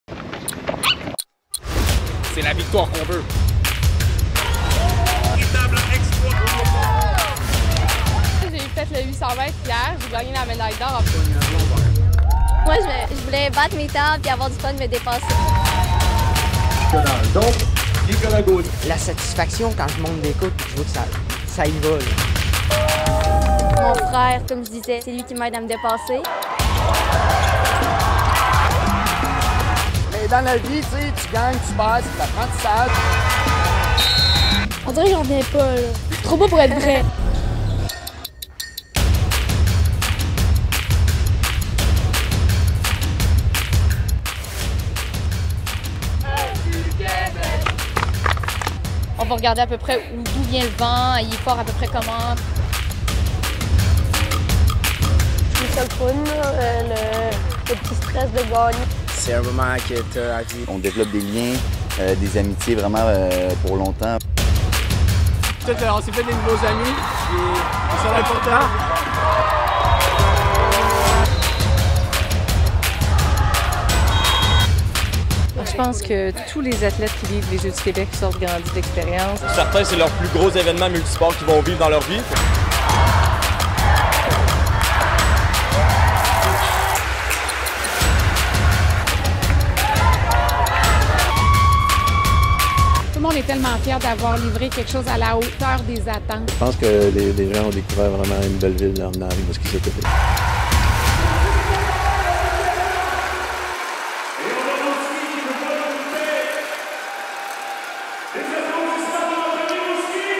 résumé des jeux du Québec à Rimouski 2023
Montage d'un résumé de la semaine des Jeux du Québec à Rimouski en 2023.